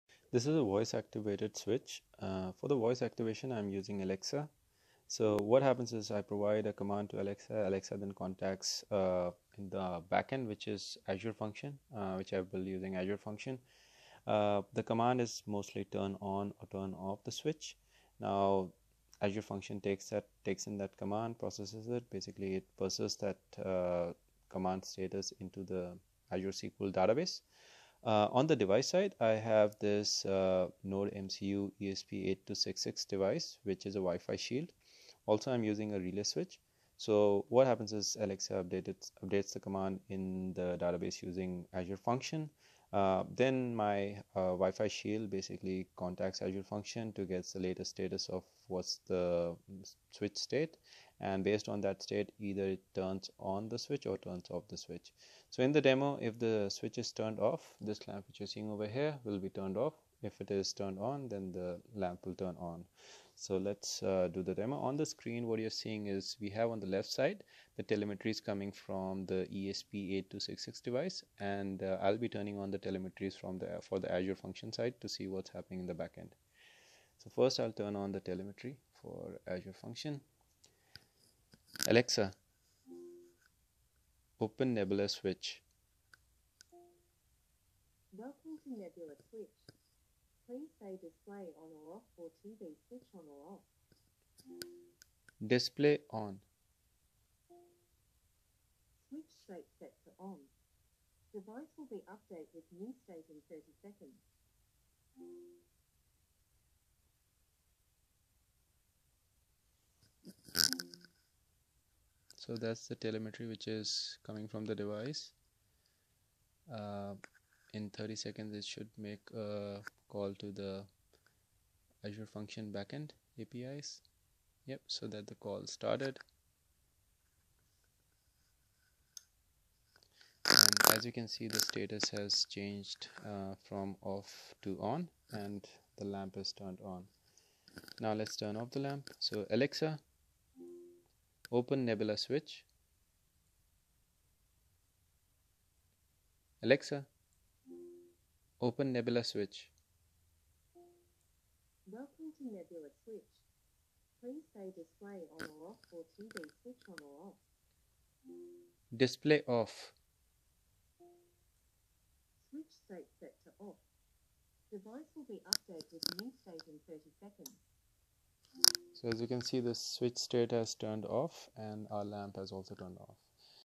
Alexa Voice Activated IoT Switch
Nebula switch is a voice activate IoT switch which is integrated with Alexa echo dot. The technologies used are
1. ESP8266 Node MCU - IoT Switch
2. Azure Functions, Azure Application Insight - Backend API
3. Alexa Echo dot and Alexa Skill